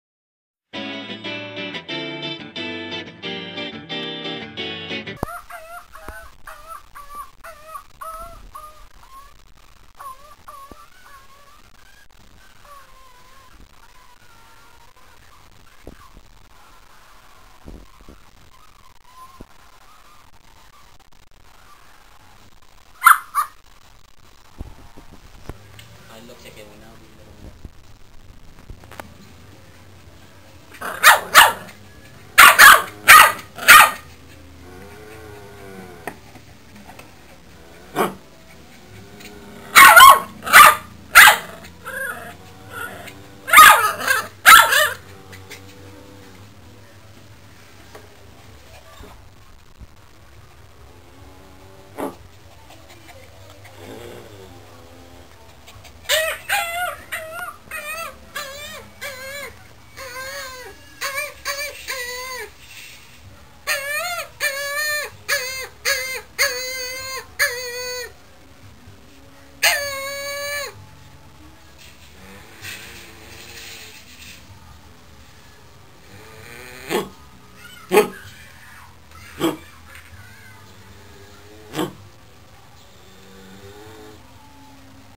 my bro and dyosa.wmv
my bro teases our dog, dyosa..